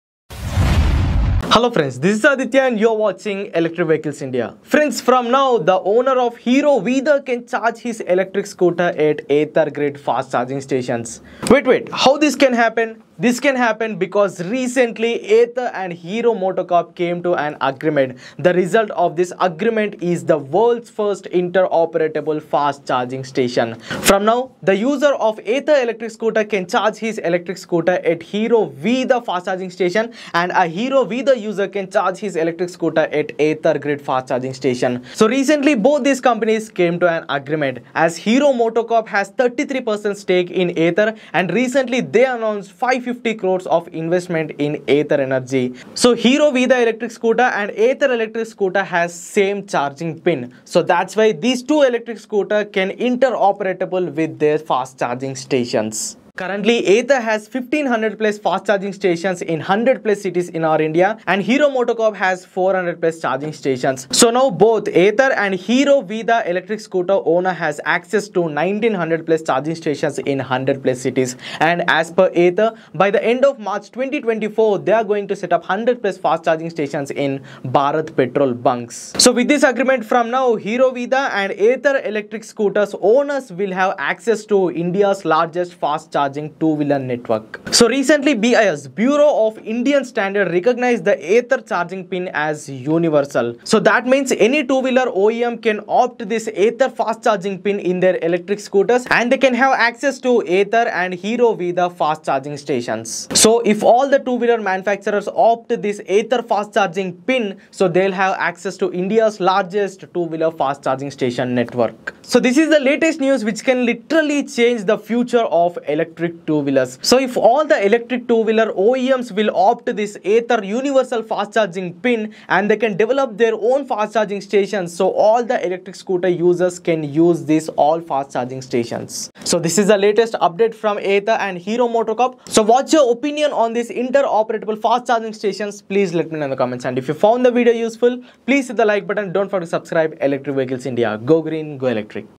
Ather 🤝 Hero Motocorp | Hero Vida To Use Ather Fast Chargers | Electric Vehicles India
Two of India’s leading manufacturers in the automotive industry, Hero MotoCorp and Ather Energy, have joined forces to establish an innovative fast-charging network for electric two-wheelers in the country. The partnership has resulted in a combined network of over 1,900 fast-charging points that are spread across 100 cities in the country. This collaboration will enable seamless usage of both networks for EV users, providing them with greater convenience and accessibility.

Hero Vida